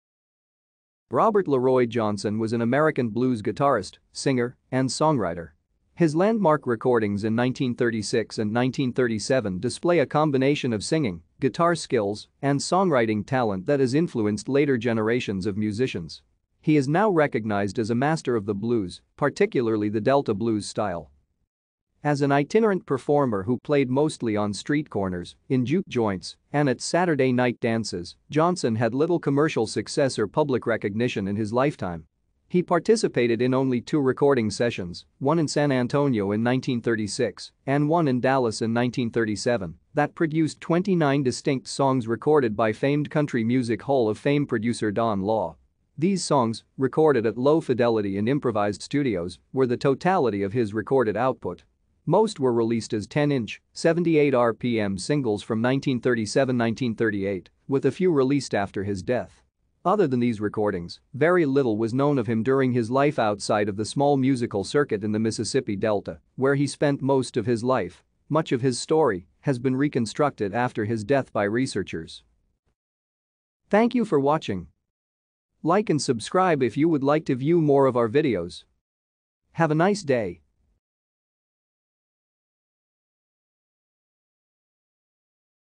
Robert Johnson
Robert Leroy Johnson  was an American blues guitarist, singer, and songwriter. His landmark recordings in 1936 and 1937 display a combination of singing, guitar skills, and songwriting talent that has influenced later generations of musicians.  He is now recognized as a master of the blues, particularly the Delta blues style.